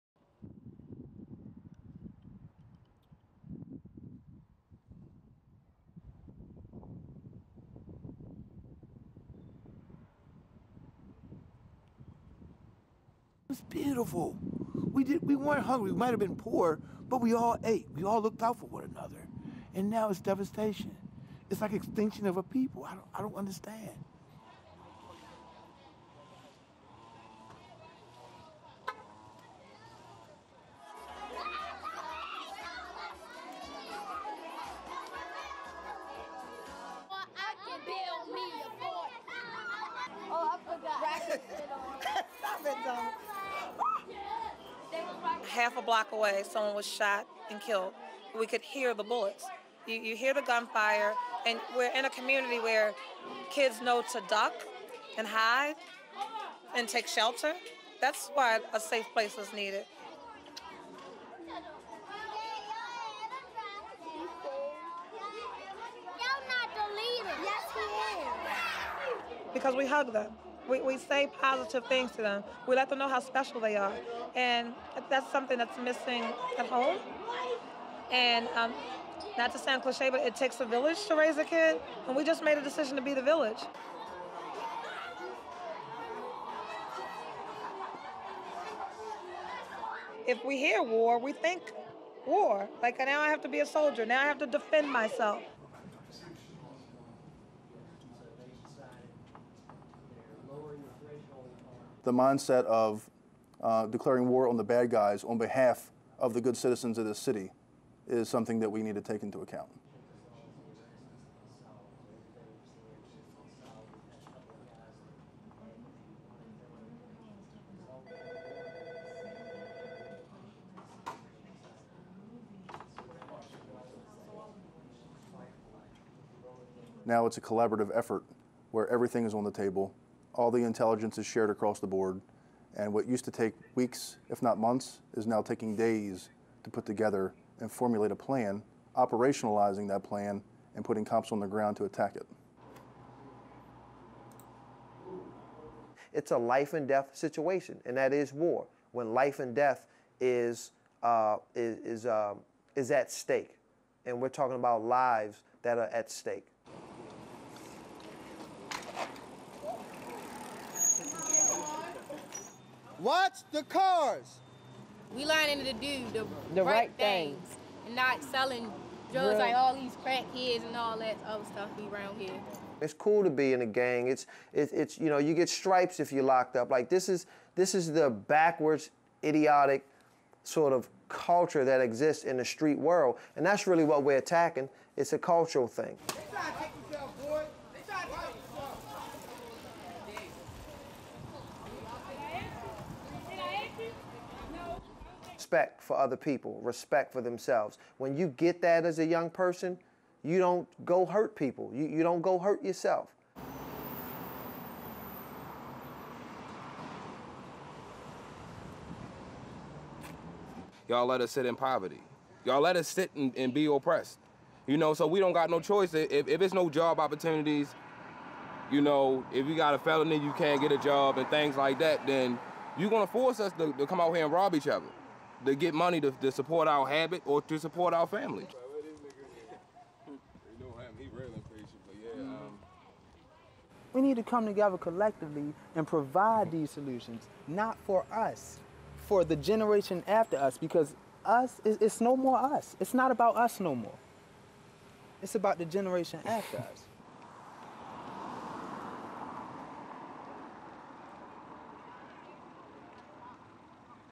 Baltimore activists try to end the cycle of violence
Baltimore in the United States has a reputation for violence, with more than two hundred murders so far this year.
With the numbers of homicides spiralling, community leaders are mobilizing to protect the city's children.

STORY-LINE
Of all the neighbourhoods in the city, West Baltimore has been disproportionately affected by the violence this summer.
Its corners are flanked by whole blocks of vacant buildings, often wrapped in yellow police tape. The Western District has seen 43 homicides this year _ far more than any other district in the city.
In April, officers arrested Freddie Gray, a 25-year-old black man.
He suffered a critical spine injury in the police transport van and died seven days later. His death spawned protests that gave way to violence and looting, and the city's homicide rate began to skyrocket.
The police commissioner lost his job, and half a dozen officers were indicted in connection with Gray's death, but the violence continued.
Derek Bowden, a Baltimore Community Activist stands outside a row of abandoned houses in the city. He mourns for the city he once knew.
"It was beautiful.  We might have been poor, but we all ate, we all looked out for one another.  And now it's devastation.  It's like extinction of a people, I don't understand."
The challenges the city faces has produced two very different responses.
A converted laundromat in West Baltimore, just blocks from where Gray was arrested, has been transformed into a safe zone for local children.
There are only three rules at the Kids Safe Zone: Sign in, clean up after yourself, and read for 15 minutes before playing with the toys stacked around the space.
Ericka Alston launched the centre in the poor, crime-riddled neighbourhood in response to the violence that followed the death of Freddie Gray.
"Half a block away someone one was shot and killed. We could hear the bullets,  you hear the gunfire and we're in a community where kids know to duck and hide and take shelter.  That's why a safe place was needed" she explains.
"Because we hug them, we say positive things to them. We let them know how special they are.  And if that's something that is missing at home and�not sound cliched, but it takes a village to raise a kid.  We just made a decision to be the village." Alston adds.
Across town, in a high-rise conference room downtown, Baltimore police are working on a very different message.
Last month, the Police Department launched the War Room, a physical and metaphorical effort to combat the spike in killings.
Some community leaders such as Alston question the language chosen by the police
"If we hear war,  we think war�like now I have to be a soldier. Now I have to defend myself" says Alston.
Inside, the War Room is lined with monitors blinking with closed-circuit TV footage from around the city.
Operation commander Lt. Col. Sean Miller sits at a table in the centre of the room.
He calls roll at 3 p.m. daily, checking in with each federal agency that makes up the task force: the FBI; the Drug Enforcement Administration; the Bureau of Alcohol, Tobacco, Firearms and Explosives; the U.S. Marshal Service; and the Secret Service.
The Police Department's homicide unit, the State's Attorney's office and other law enforcement agencies also are represented.
In total, Miller said, more than a dozen organizations are involved, and the War Room is the sole assignment for about 200 people.
Lt. Col. Sean Miller, Baltimore Police, War Room commander defends the use of the word 'war'.
The 300 Men March, is an organization dedicated to promoting peace and empowerment.